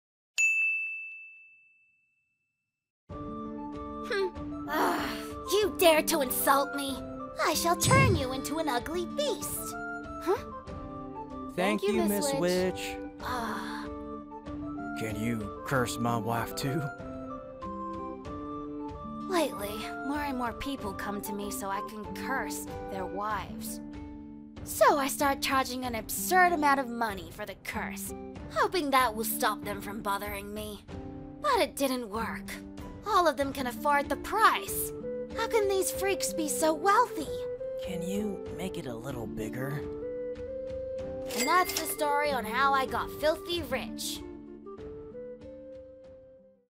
Witch's Curse Turn Farmer Wives Into Monster Girls | Bonny comic dub
Suffering from success.